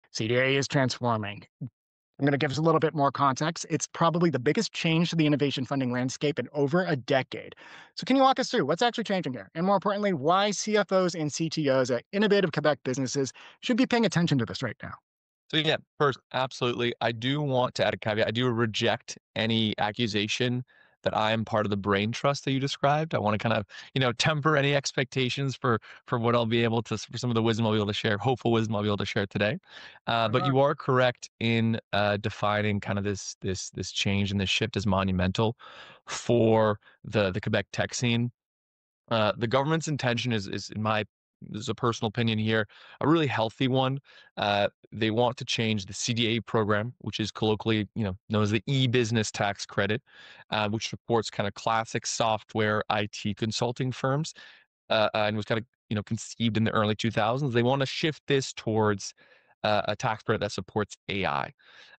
Quebec's CDAE-IA: The biggest change in innovation funding in more than a decade
Why should Quebec-based CFOs and CTOs in Quebec care about CDAE-IA? As the province overhauls its popular e-business tax credit (legacy CDAE) to reward "significant AI integration" into R&D processes (enter, CDAE-IA), it's an opportunity for businesses to modernize and enhance their approaches to funding product development and actually executing R&D.